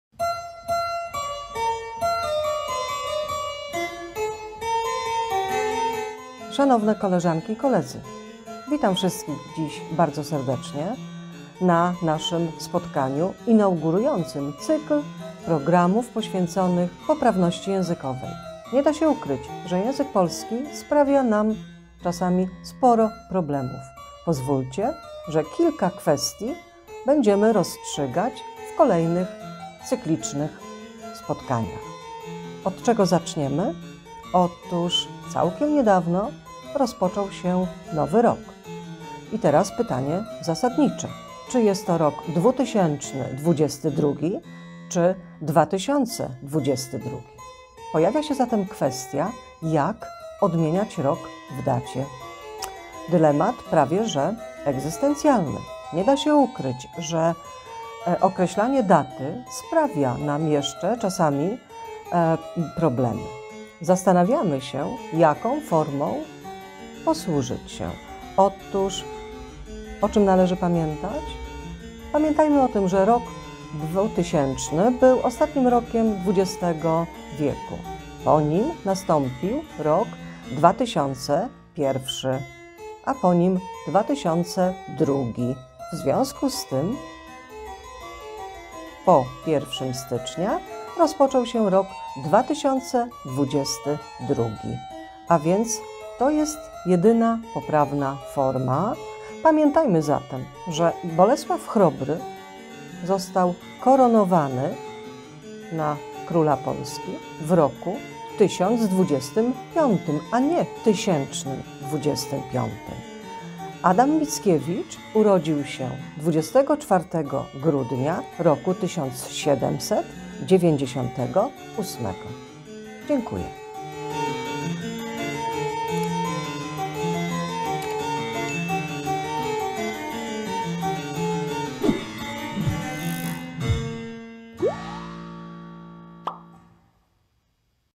Od słowa do słowa - #1 Który mamy rok?
Iwona Rostankowska, nauczyciel konsultant z Kujawsko-Pomorskiego Centrum Edukacji Nauczycieli w Bydgoszczy, wyjaśnia różne zagwozdki związane z językiem polskim.
Montaż i realizacja: Piotr Szczepańczyk